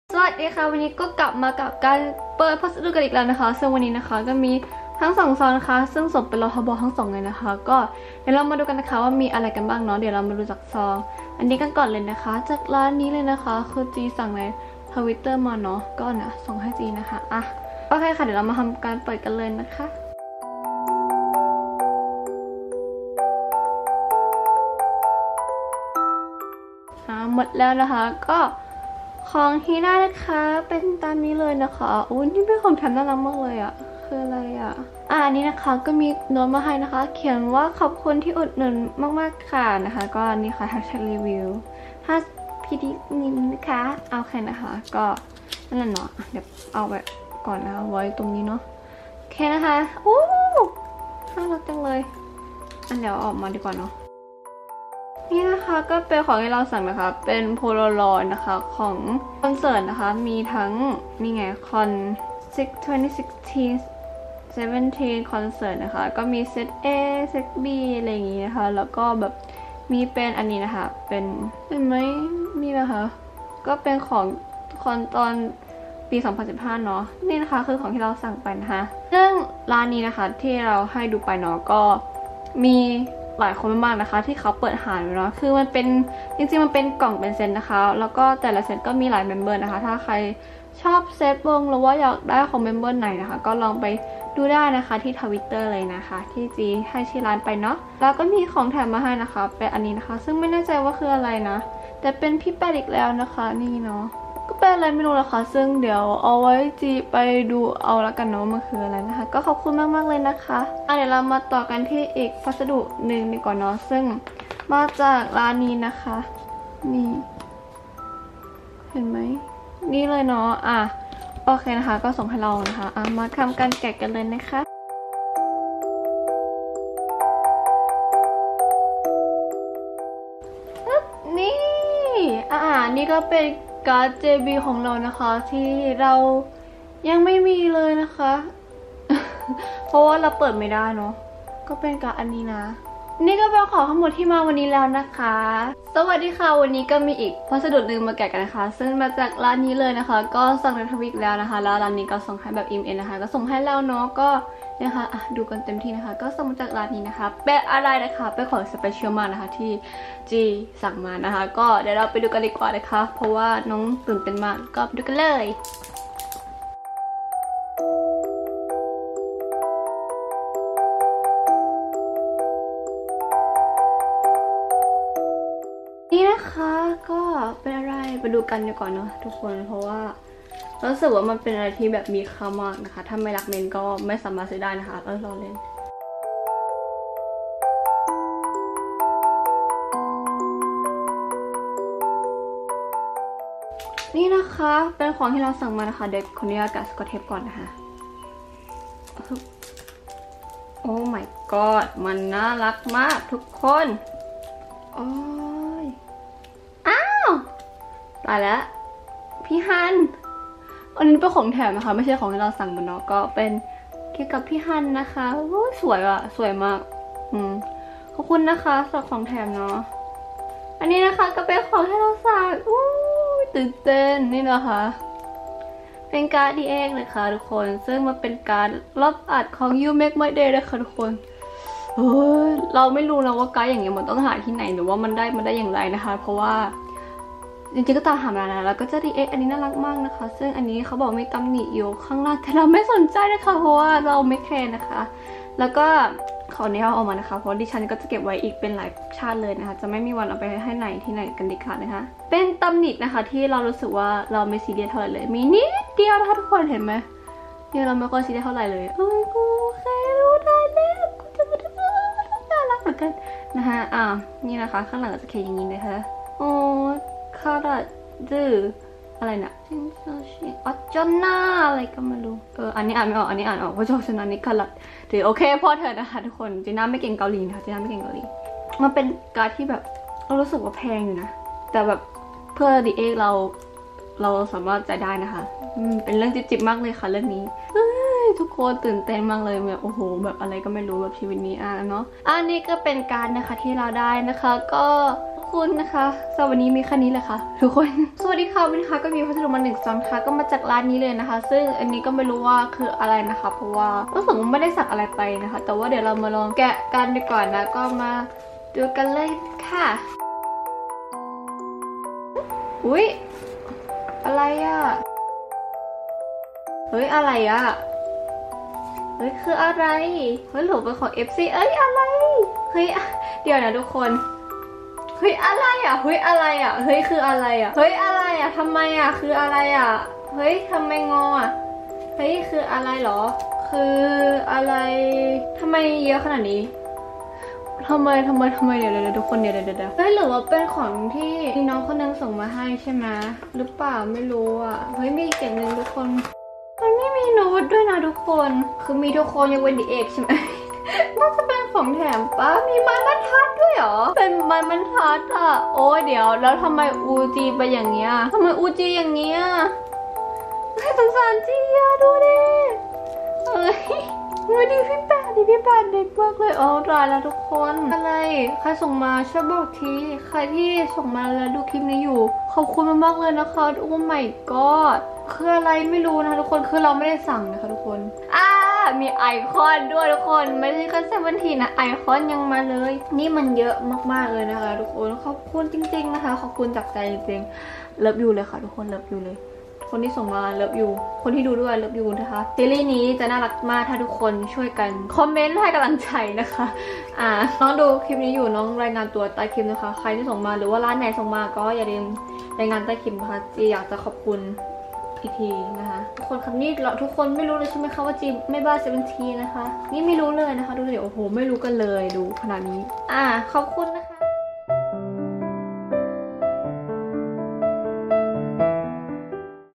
[ HAUL ] มาๆแกะพัสดุการ์ดทั้งหมดในเดือนตุลาคม👻 | GinaJJ 💕
🐸🌻💙 Here come a Package 📦
มาแล้วจ้าาคลิปแกะการ์ดใหม่ หวังว่าเพื่อนๆยังไม่เบื่อนะ (หรือเบี่อแล้ว) อย่าเบื่อเค้าเลยย😂😂
หวังว่าจะชอบกันนะ ดูเพลินๆก็ได้ค่าาา~💗

เดี๋ยวเรามาทำความรู้จักกับจีกันดีกว่า🖤

ชื่อเล่น : จีน่า  / Gina / 지나
เกิดปี : 2545 / 2002
อยู่ : ประเทศไทย / THAILAND 🇹🇭

สามารถเม้นคุยกันได้ค่า😁